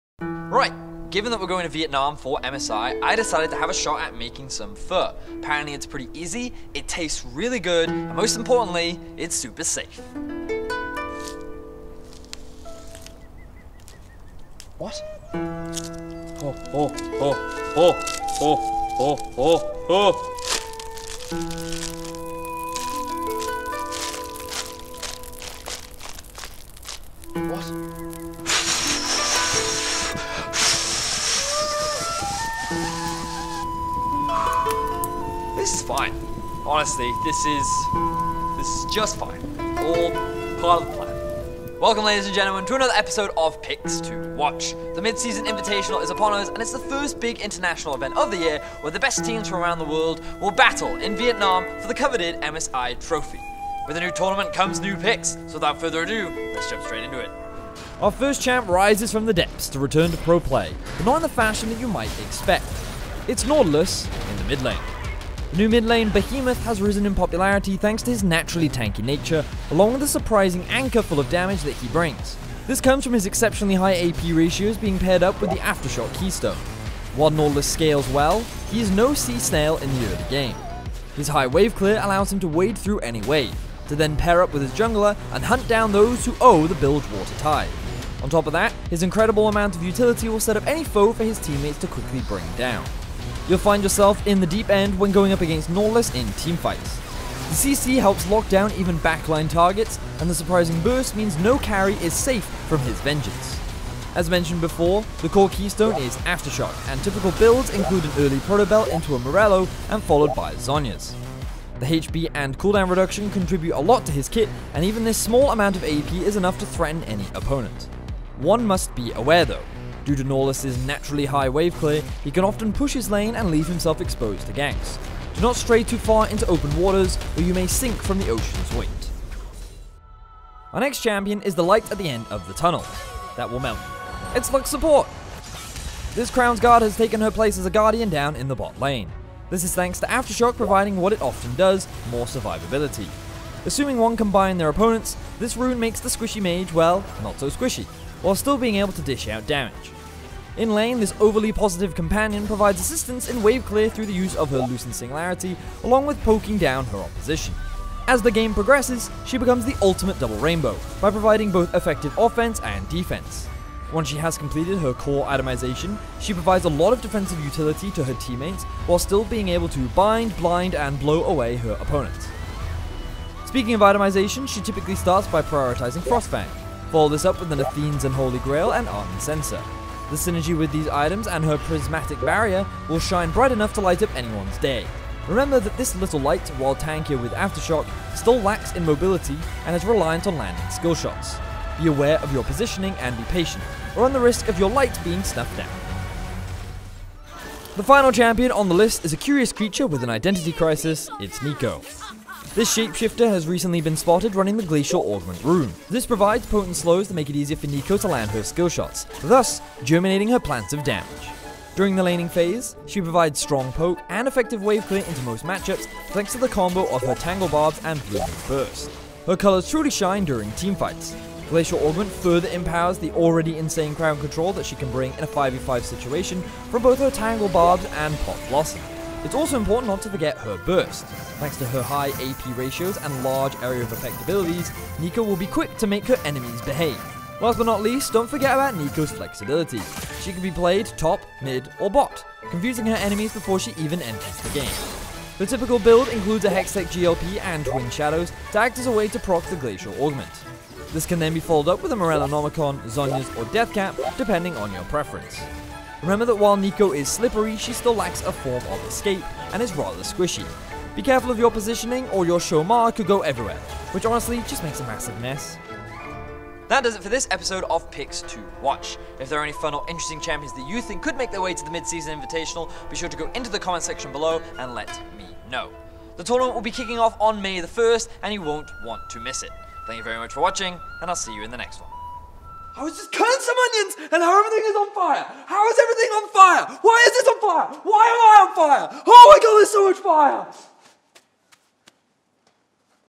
Vedius' Picks to Watch: MSI 2019
Welcome to another episode of Picks to Watch, where Vedius looks at some of the picks that you might see during MSI2019!